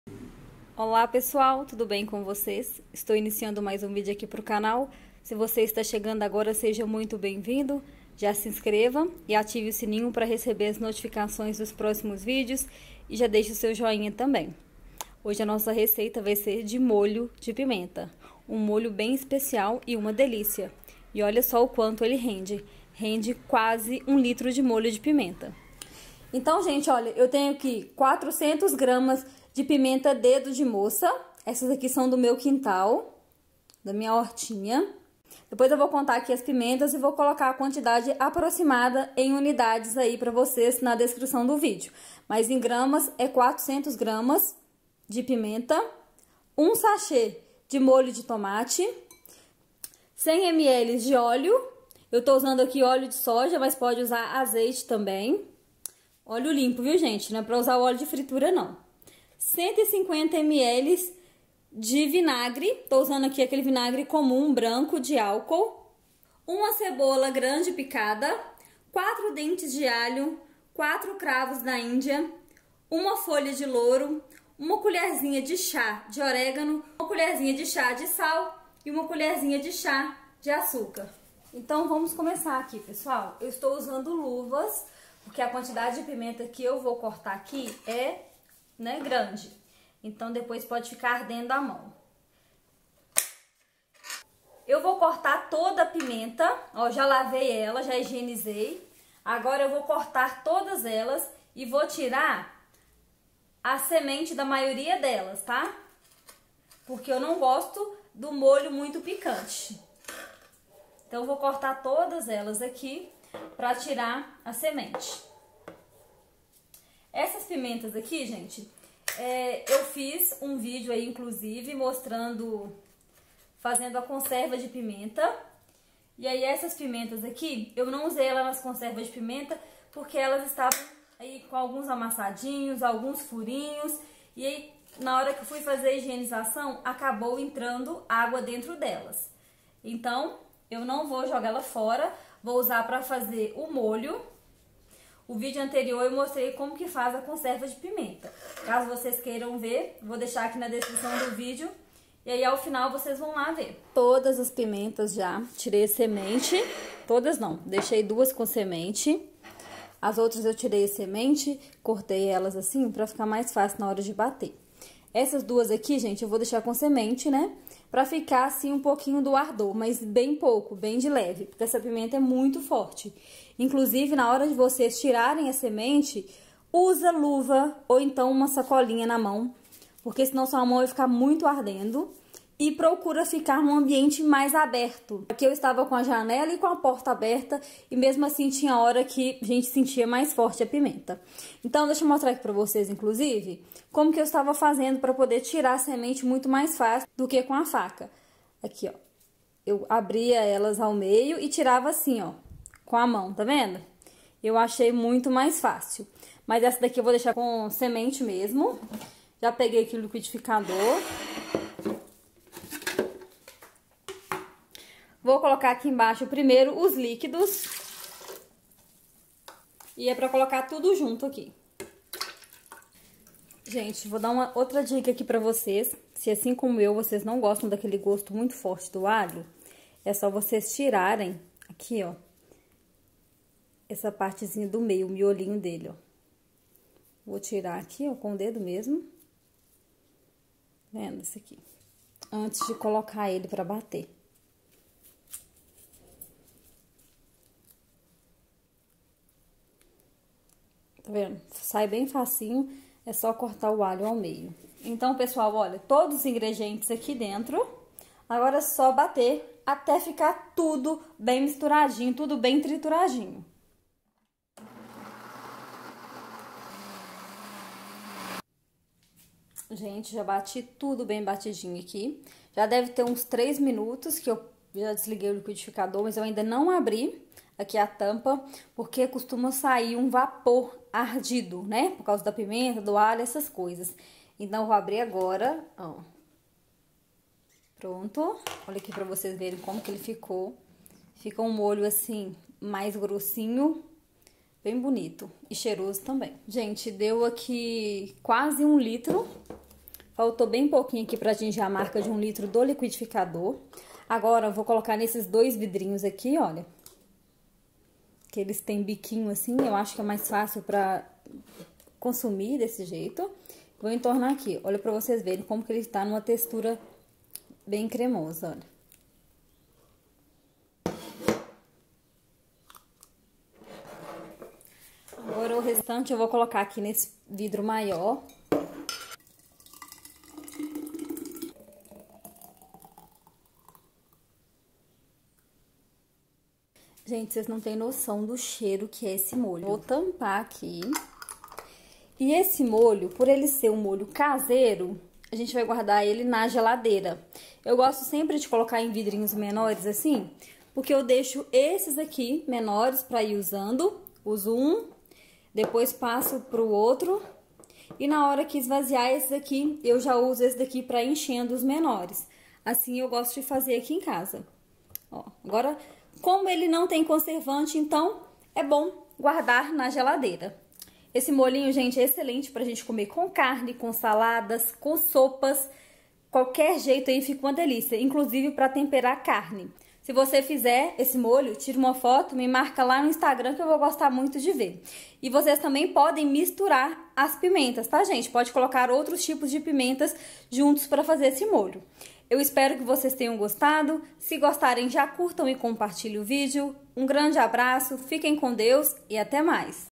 COMO FAZER MOLHO DE PIMENTA
INGREDIENTES:
400Gr de Pimenta
1 Sache de Molho de Tomate
100ml de Óleo
150ml de Vinagre
1 Folha de Louro
4 Cravos da Índia
4 Dentes de Alho
1 Colher de Chá de Açúcar
1 Colher de Chá de Sal
1 Colher de Chá de Orégano